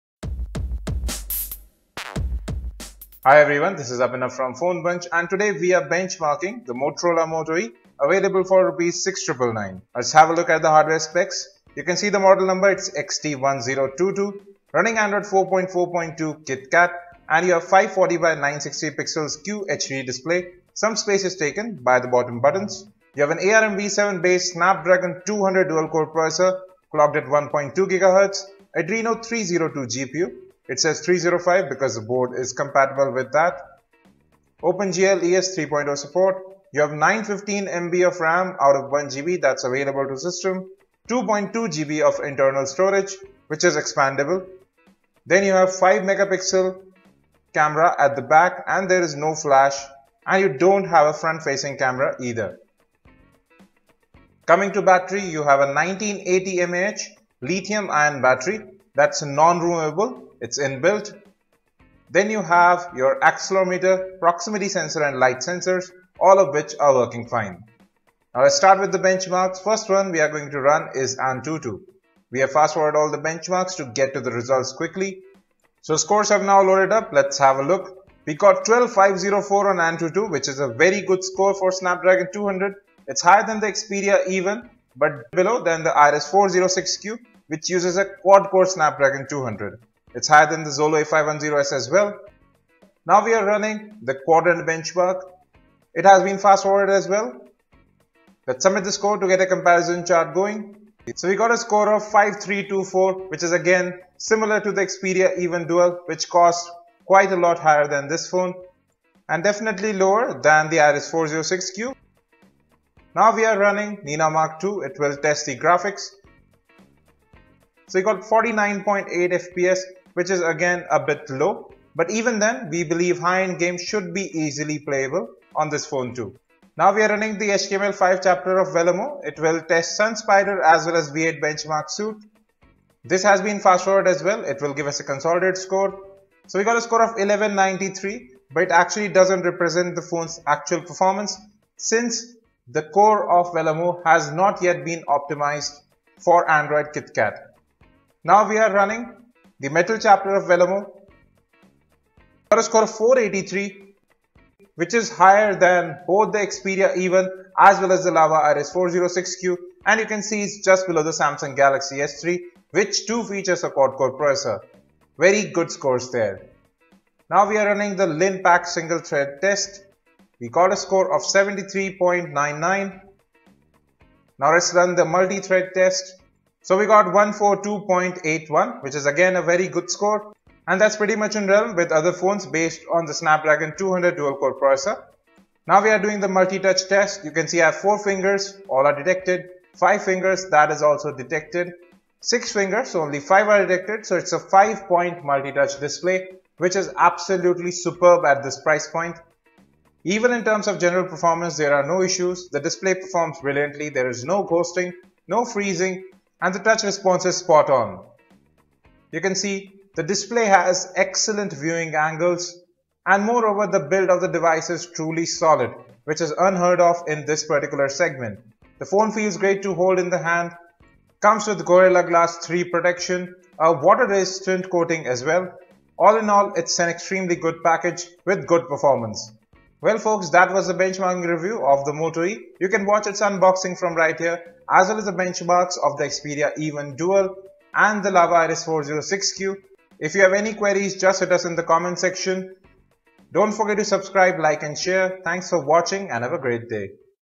Motorola Moto E Benchmarks
Benchmarks of the Motorola Moto E Dual SIM on AnTuTu, Quadrant, NenaMark 2, Vellamo and Linpack alongwith a look at general performance and touch response.